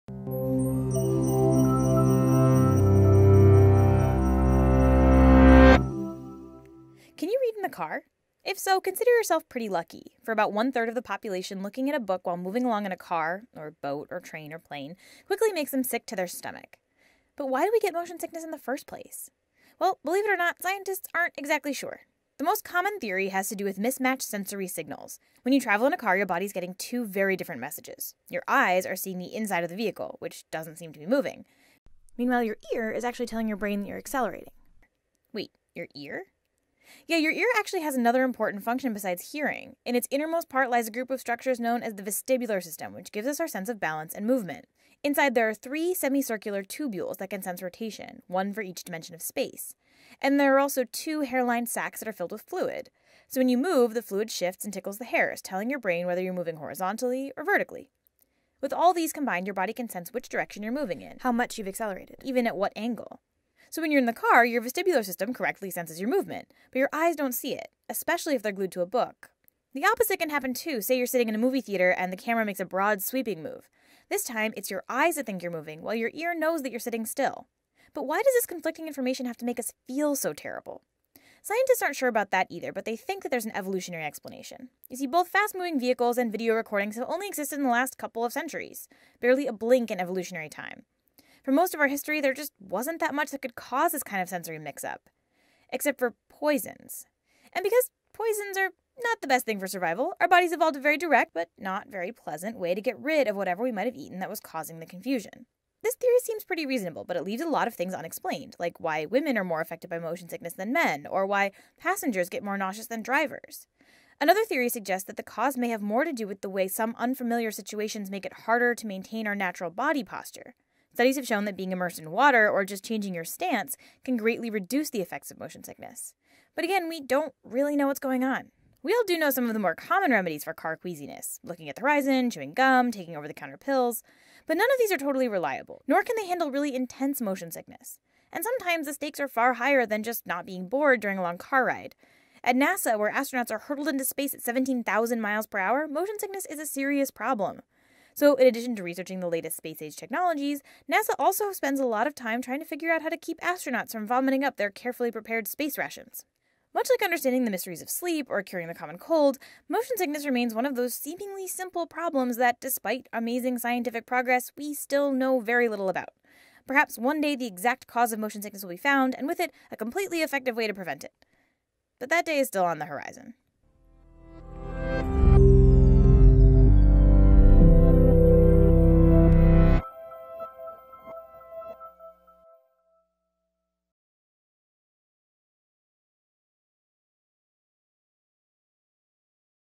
Tại sao chúng ta mắc chứng say tàu xe? | Nhà Thuốc FPT Long Châu
Nguồn: TED-Ed

Nguyên nhân gây say tàu xe. Say tàu xe là cảm giác hình thành khi chuyển động cảm nhận trong tai, chuyển động cảm nhận ở mắt và cảm nhận ở cơ bắp xung đột với nhau. Khi đó, não bộ không thể xử lý tất cả các tín hiệu xung đột này, gây cho cơ thể cảm giác chóng mặt và buồn nôn.